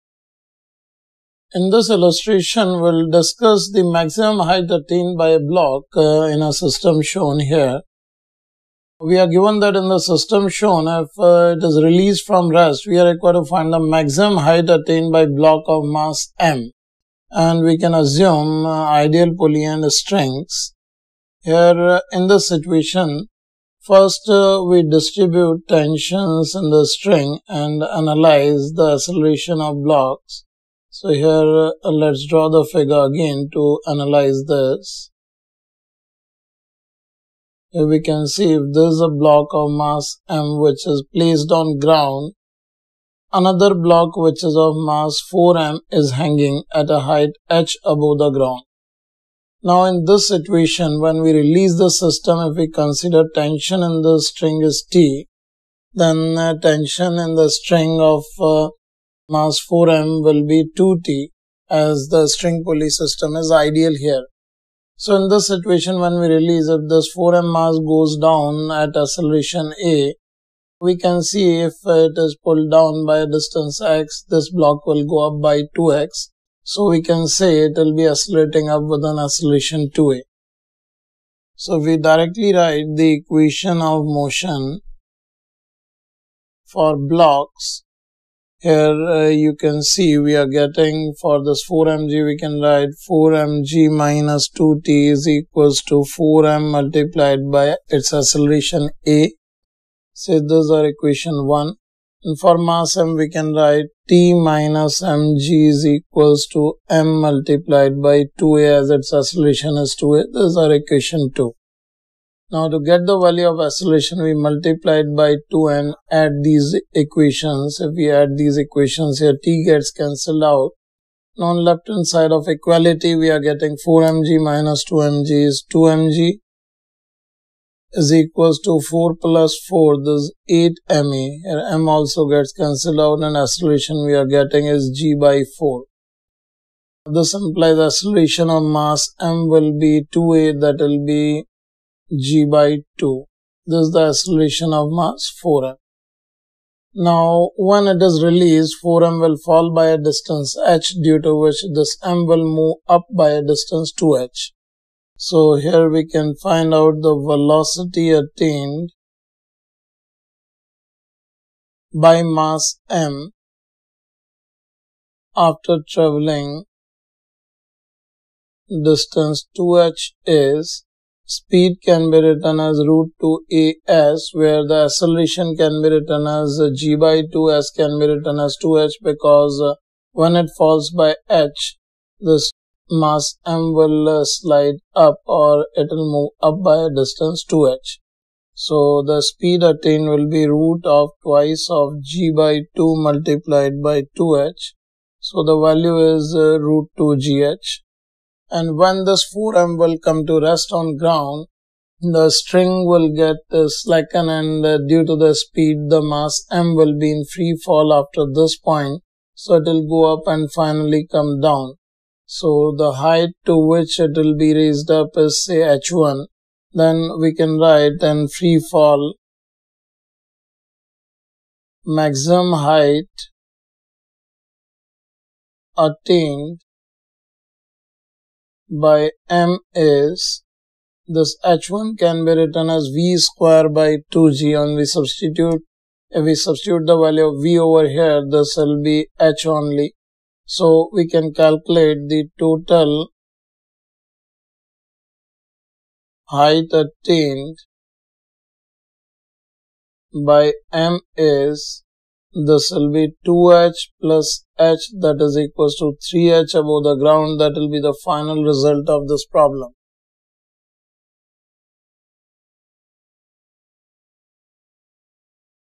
Adv Illustrations – Maximum Height Attained by a Block | Newton's Laws of Motion #33 for JEE Adv
Ashish Arora Sir has taken up a case of Maximum Height Attained by a Block which is important for select in IIT.